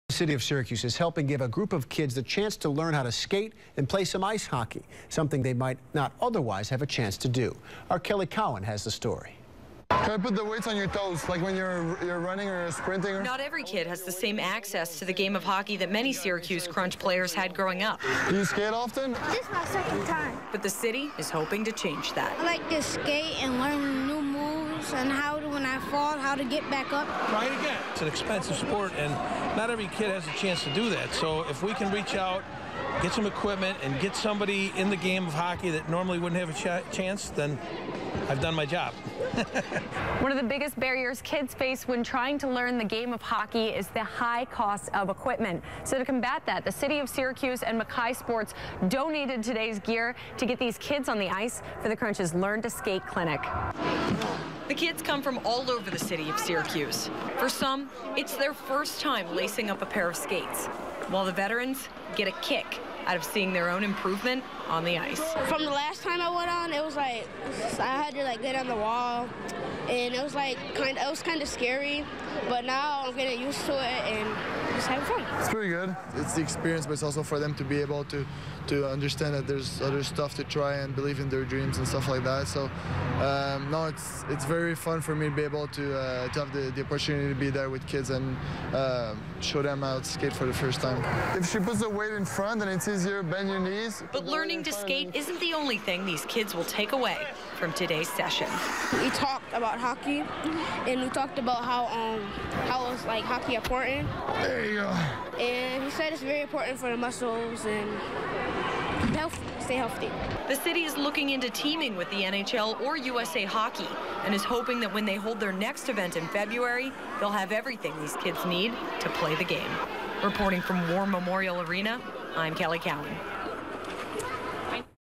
Kids get skating lesson from Syracuse Crunch players
Kids get an ice skating lesson from Syracuse Crunch players thanks to the city of Syracuse, McKie Sports and Sunny Delight.